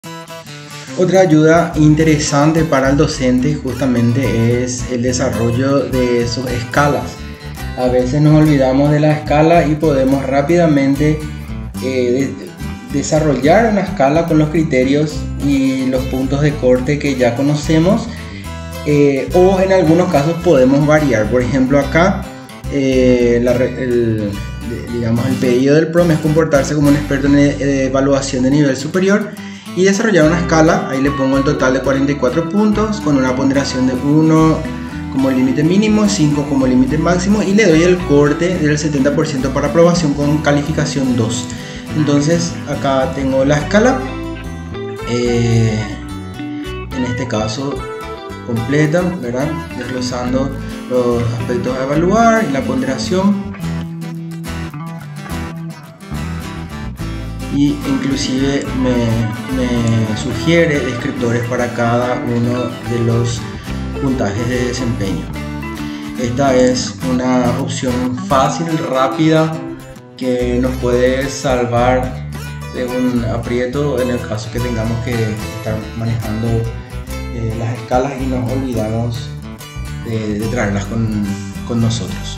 Prompt para desarrollar escala de calificación con ia
En base a criterios y puntos de corte, personaliza una escala de calificación.

Prompt utilizado:
"compórtate como un experto en evaluación de nivel superior. Vamos a desarrollar una escala de un total de 44 puntos, con una ponderación de 1 como el límite mínimo y 5 como límite máximo con un corte del 70% para la aprobación con una calificación de 2"